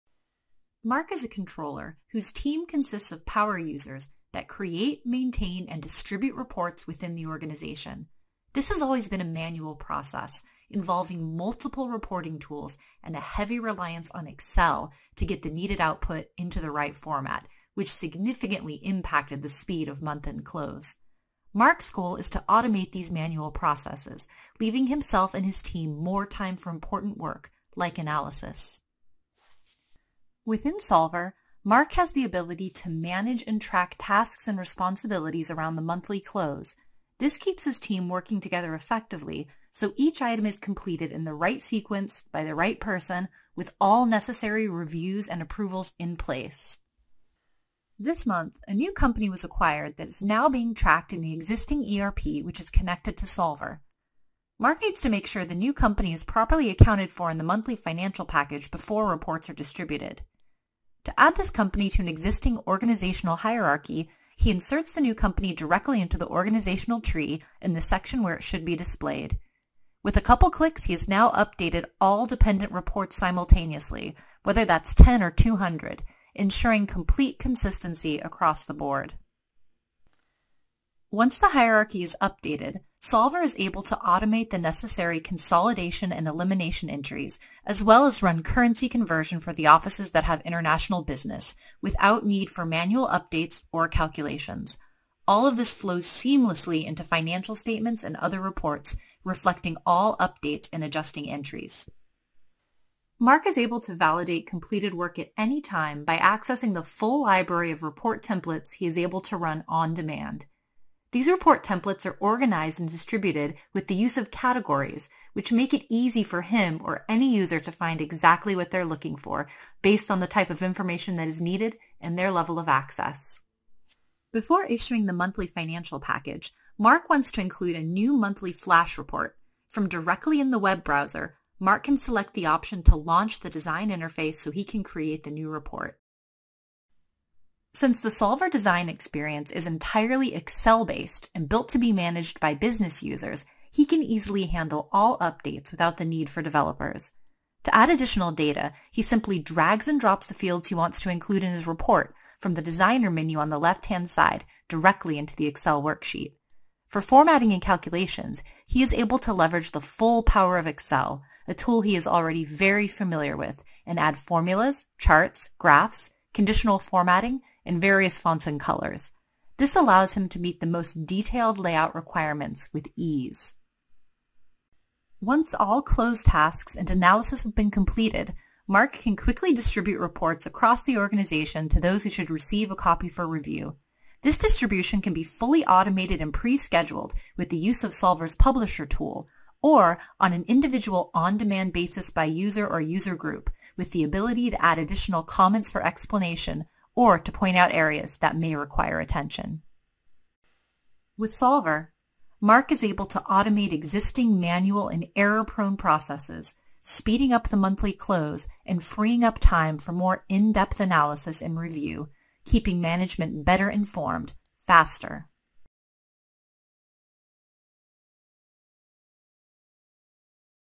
Solver Reporting and Analysis | Power User Demonstration
SOLVER REPORTING AND ANALYSIS | POWER USER DEMONSTRATION \ This brief video overview of Solver, provides a demonstration of the corporate performance management (CPM) suite, and how power users can build, maintain, and distribute reports. Solver provides a comprehensive cloud based reporting and planning solution, bringing together the best in business intelligence and corporate performance management.

BI360 software is now Solver!